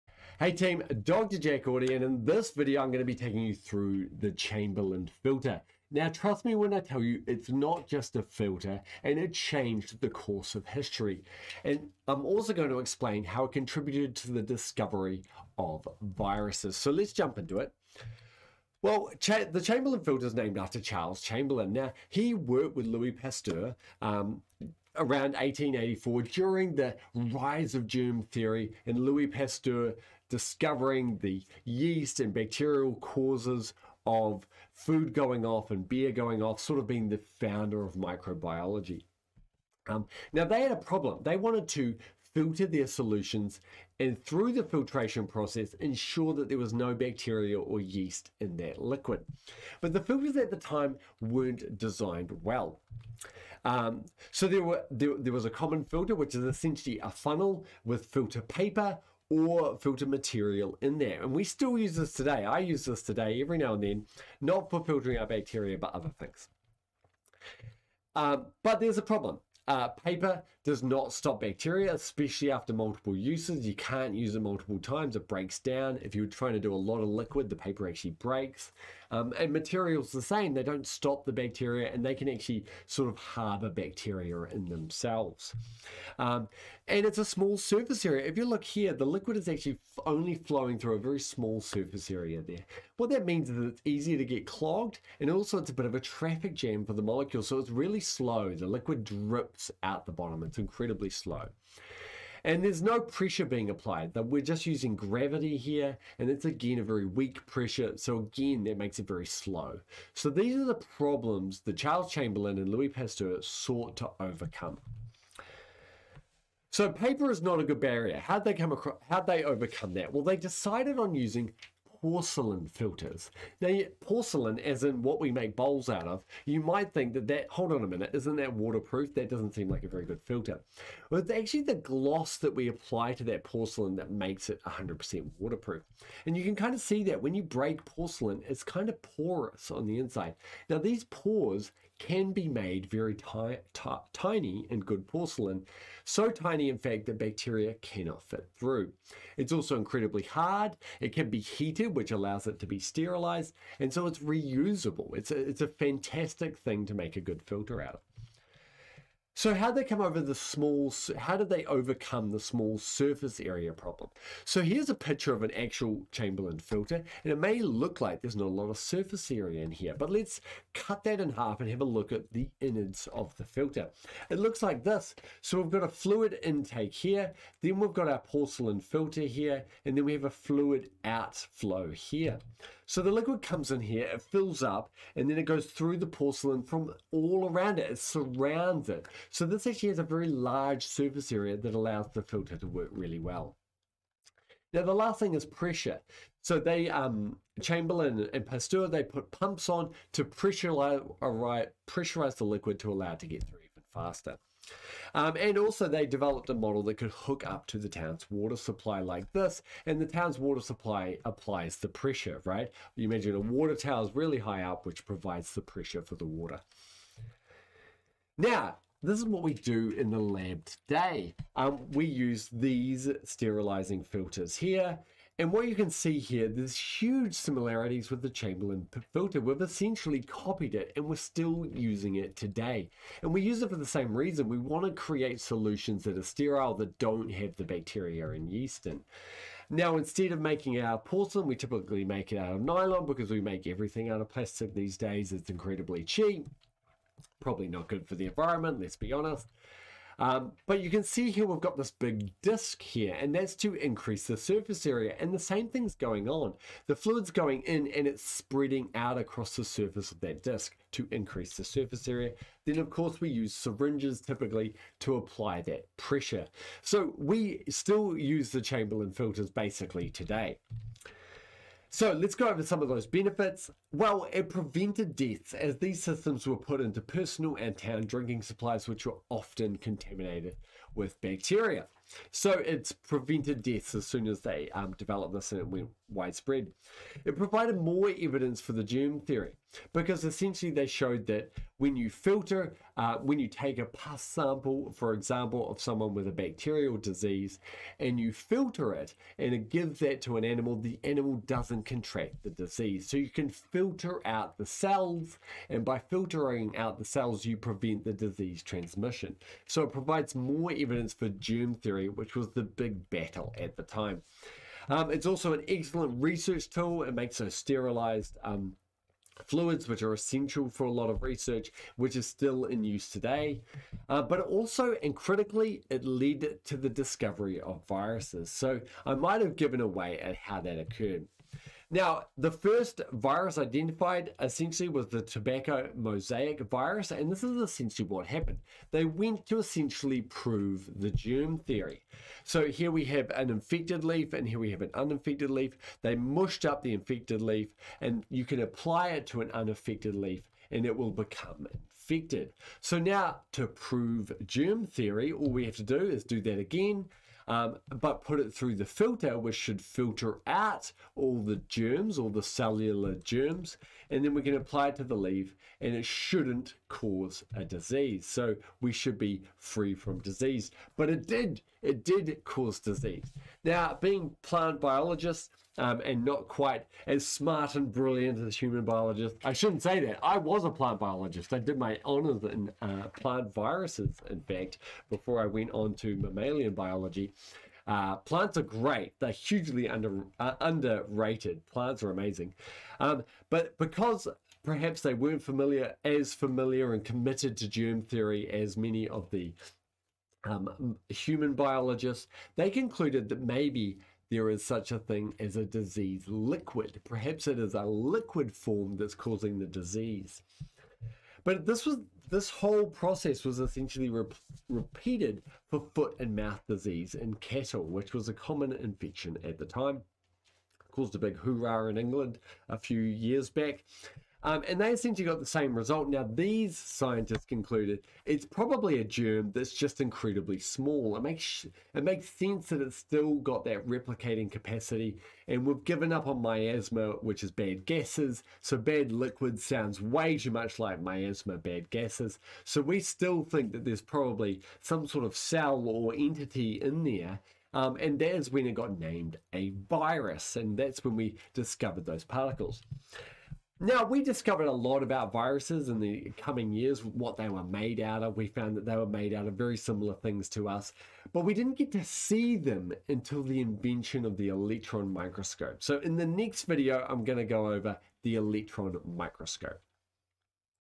How the Chamberland filter led to the discovery of viruses
In this video I discuss how Charles Chamberland and Louis Pasteur devised a simple and effective filter which was fine enough to filter out yeast and bacteria. I then discuss how this led to many brilliant things including the discovery of viruses.

Learning objectives

Explain the problems of traditional filters and how the Chamberland filter overcomes these problems.
Describe the design of the Chamberland filter and explain how it works
Explain the benefits of the Chamberland filter and how it led to the discovery of viruses